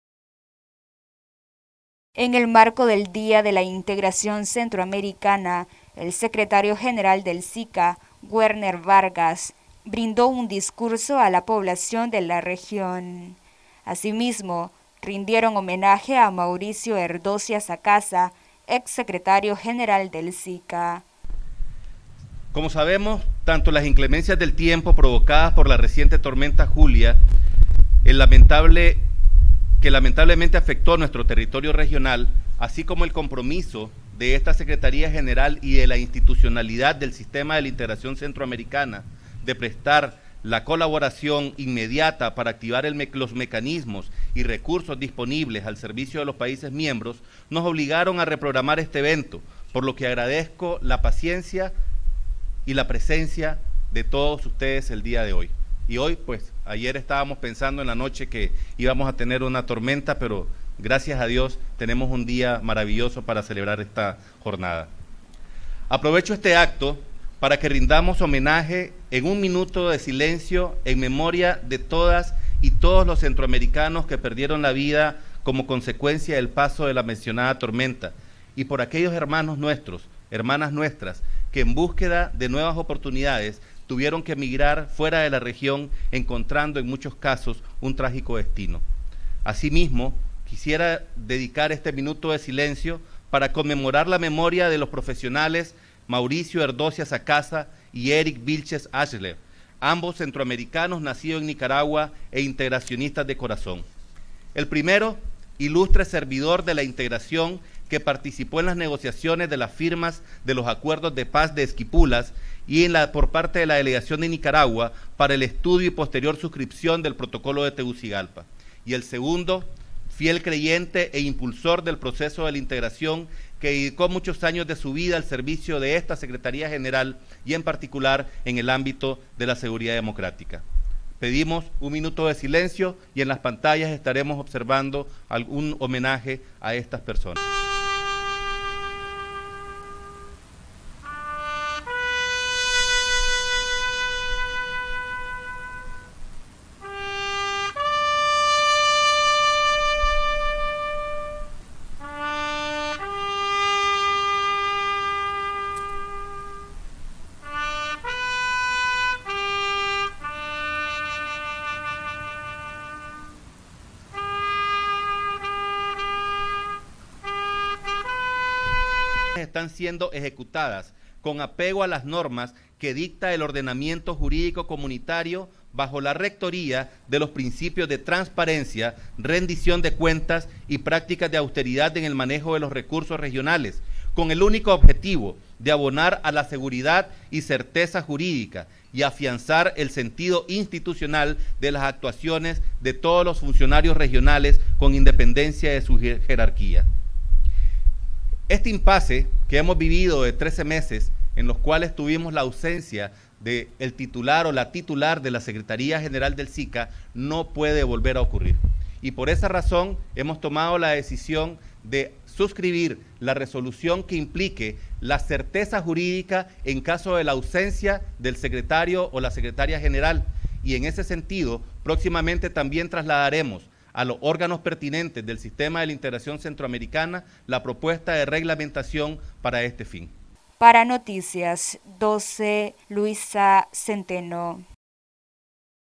Noticias12Nicaragua Realizan acto de conmemoración del día de la Integración Centroamericana
Realizan acto de conmemoración del día de la Integración Centroamericana